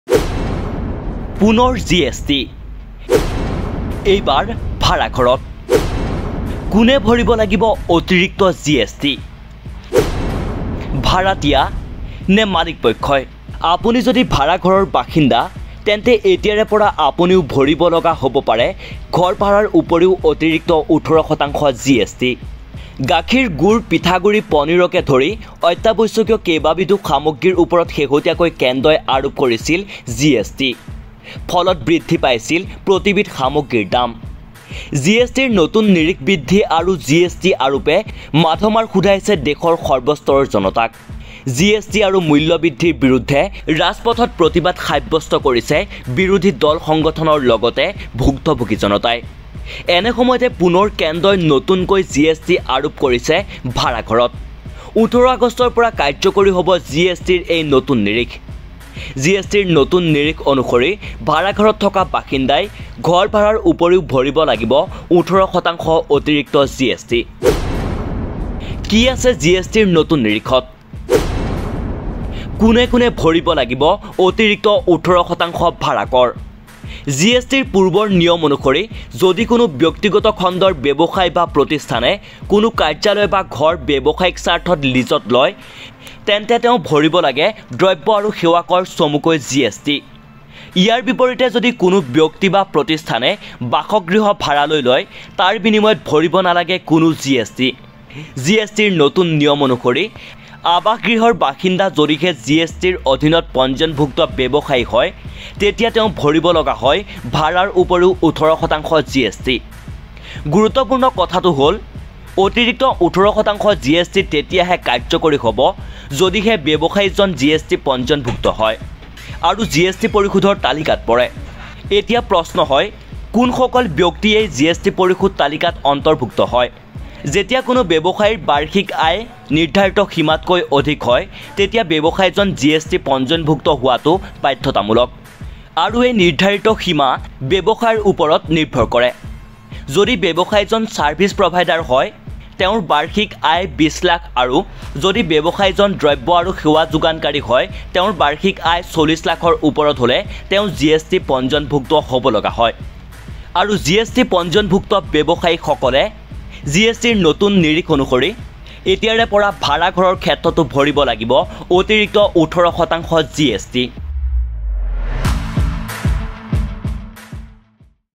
এইবাৰ ভাড়াঘৰত লাগিল GST !
এইবাৰ ভাড়াঘৰত লাগিল GST ! কোনে ভৰিব লাগিব অতিৰিক্ত  GST?